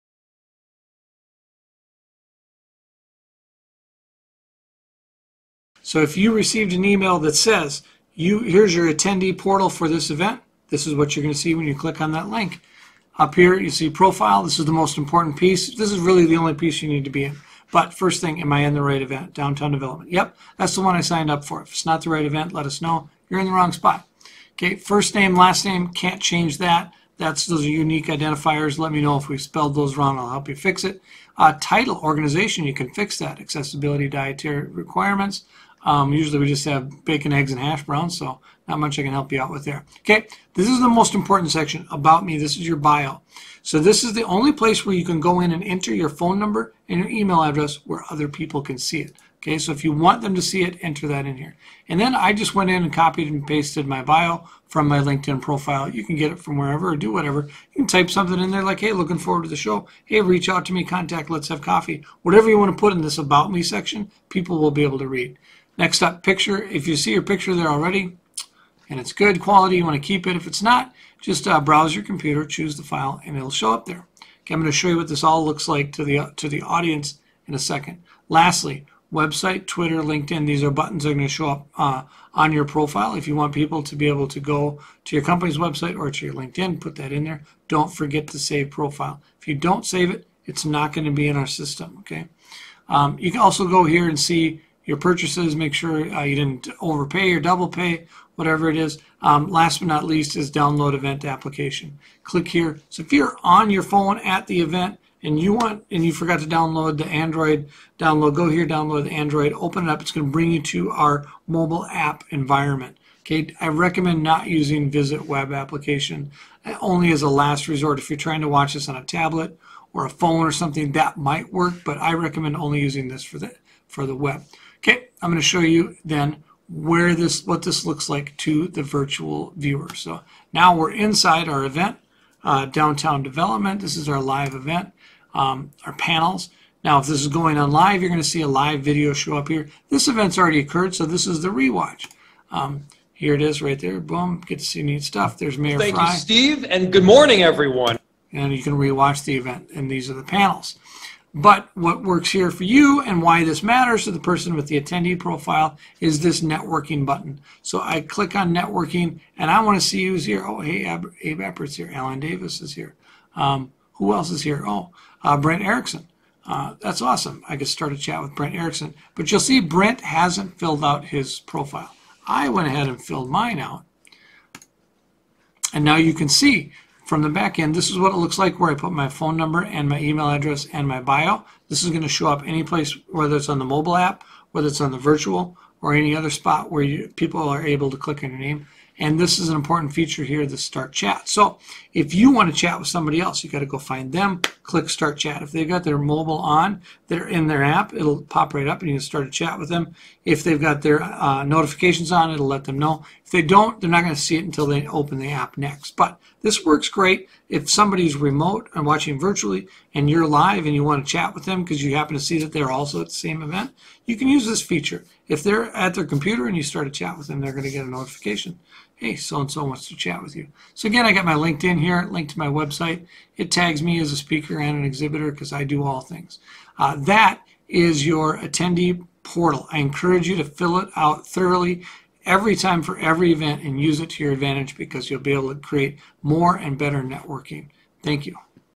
Attendee Portal Tutorial REjournals
How to enter your attendee information in PheedLoop